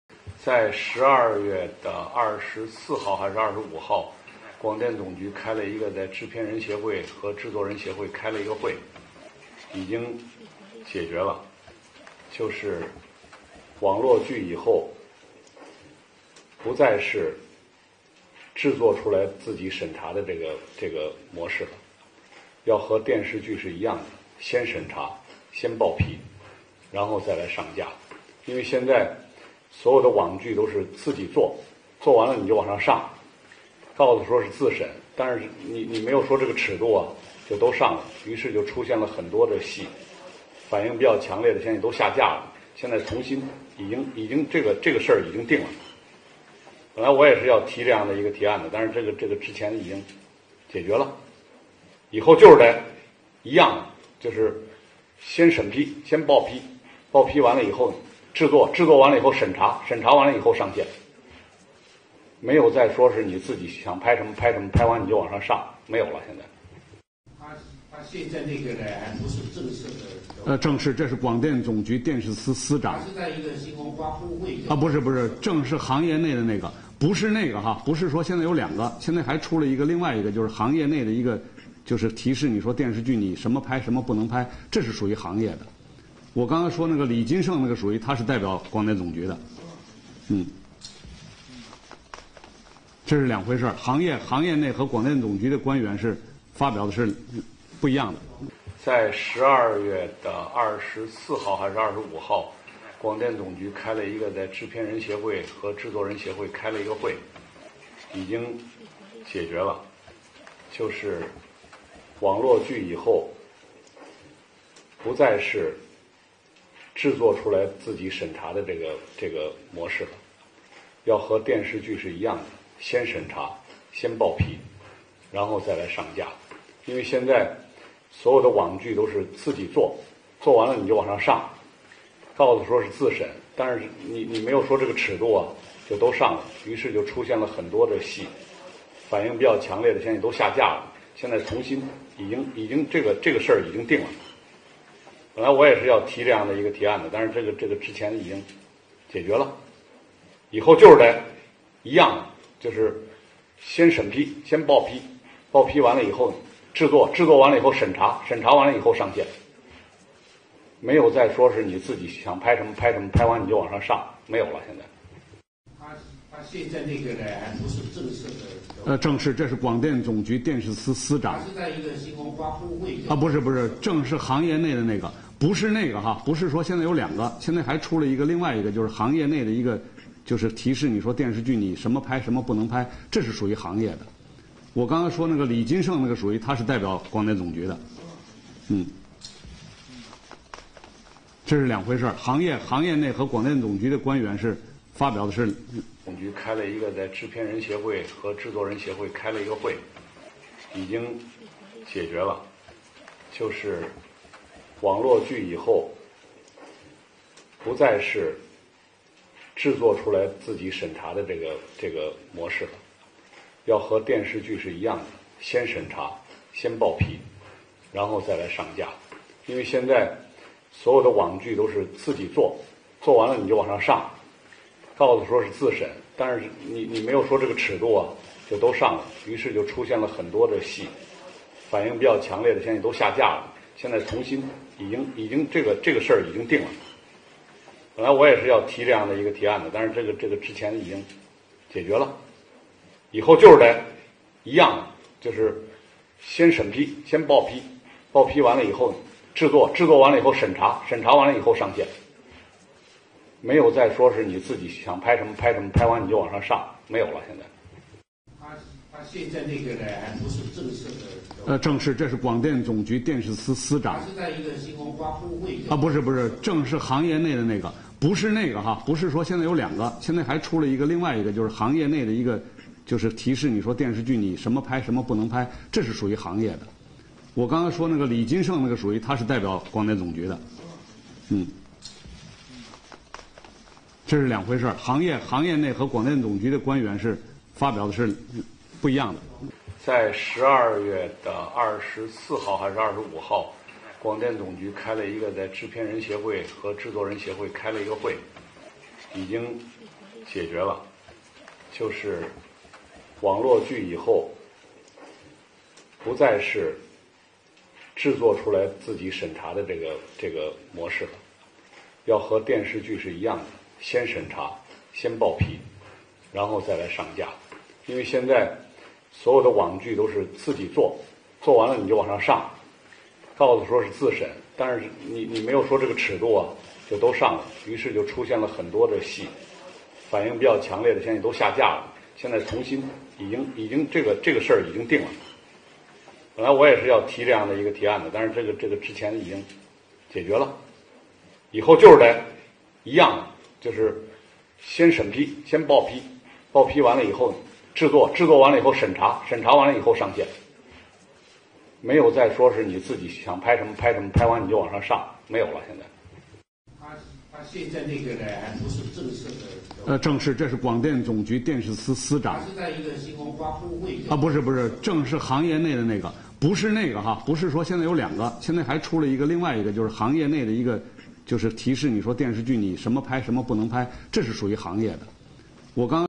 张国立：网络须有严格监管限制，不能随便拍什么就放网上
4日晚，“央视新闻”官微称，张国立在两会呼吁加强网络剧管理。5日下午，张国立发博称，自己尚未在会上发言。此事就此几经反转陷入罗生门。最.

央视称张国立要求中共强化网剧管理】 【张国力否认后央视拿出现场录像证明】 3月4日上午11点，北京会议中心，在中共全国政协十二届四次会议.